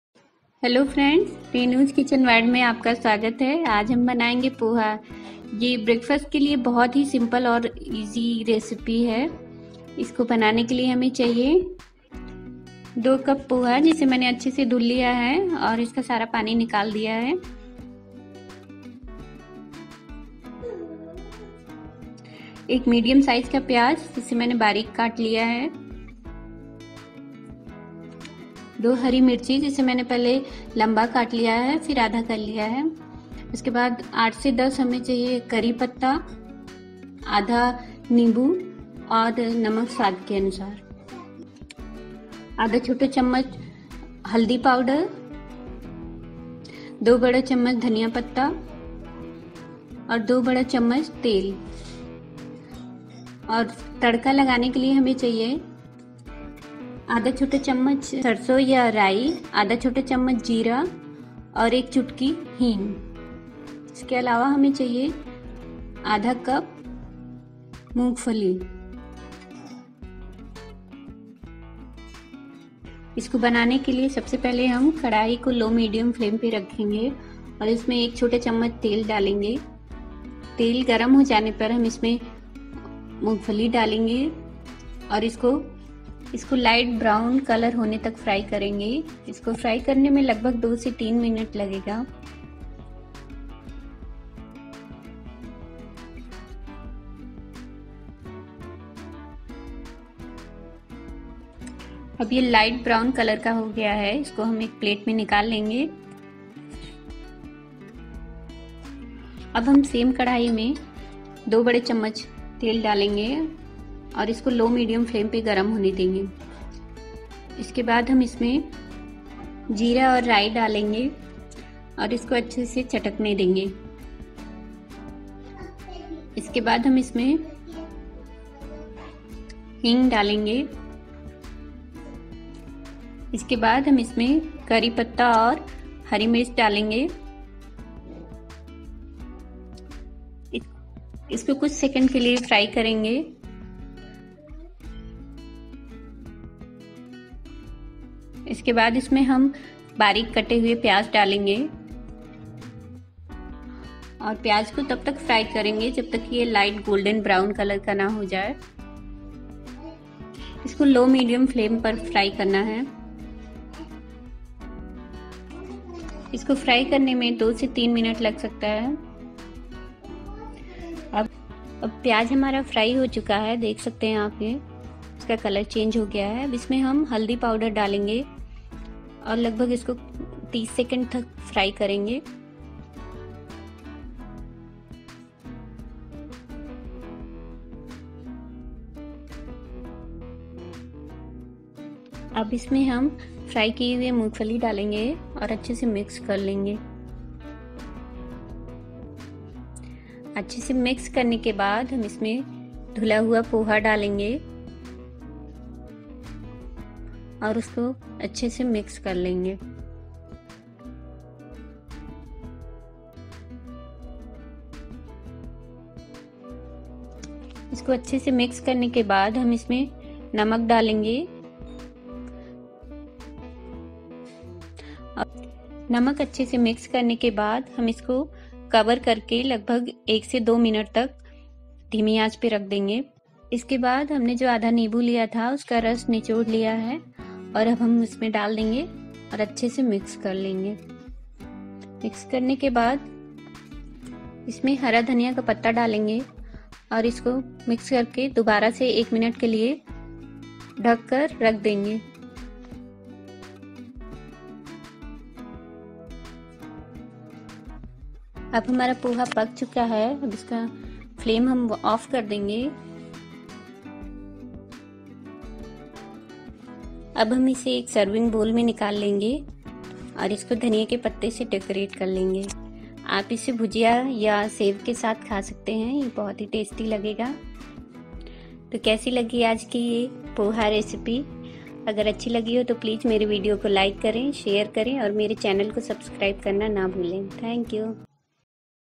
Quick & Easy Poha Recipe/Poha Recipe in Hindi/ पोहा
Quick & Easy Poha Recipe/Poha Recipe in Hindi

Ingredients:

Flattened Rice - 2 Cup
Onion - 1 Medium Size Finely Chopped
Coriander Leaves - 2 tbsp chopped
Lemon - Half

Method:

Wash the flattened rice & remove water.
Put pan on low medium flame, add little oil & then add peanuts & fry it till light brown colour approx. 2-3 minutes.
Take it out in a plate.
Add oil in same pan, let it heat.
Add cumin & mustard seeds, let it crackle.
Add asafoetida fry for few second then add chopped onions.
Fry it till turns golden brown colour approx. 2-3 minutes.
Add turmeric powder fry for approx. 30 seconds.
Now add fried peanuts & mix it well.
Add washed flattened rice & mix it well.
Add salt & mix well.
Cover it & cook it for a minute on low medium flame.
Then add juice of half lemon &  coriander leaves, mix it well.
Once again cover it & cook for 1-2 minute.
Turn off the flame.
Now poha is ready to serve.
Serve it hot in a bowl.
Garnish with coriander leaves, lemon & green chilli.

I hope you will enjoy the recipe.